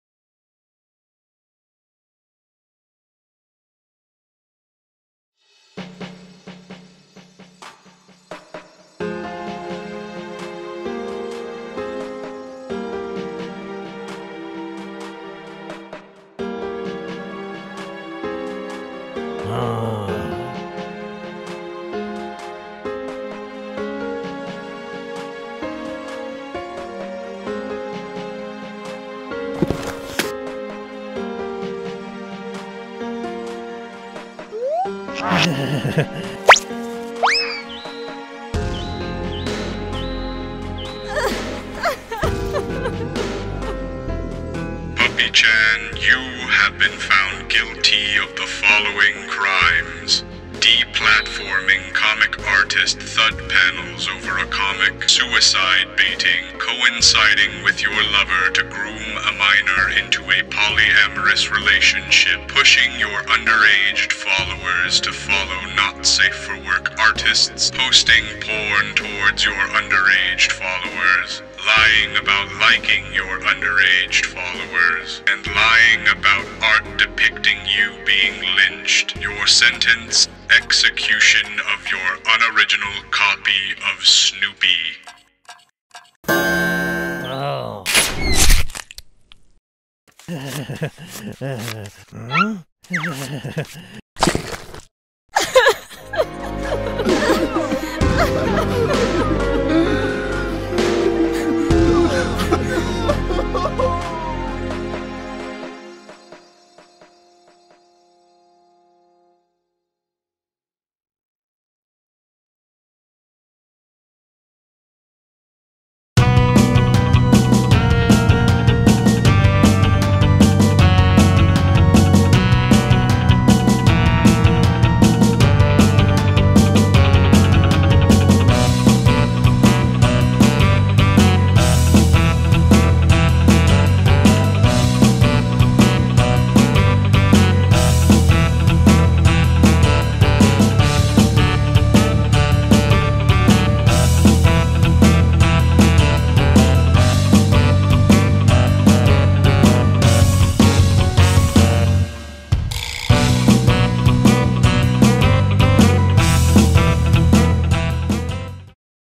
Antoons 900k Special + Trailer
This was long overdue, but it is time at last to set things straight about the drama I was in recently. Also, thank you all for the immense support you've given me over the months. I still get tons of threats from Puppychan fans, but please do not harass her. This video was made to spread awareness.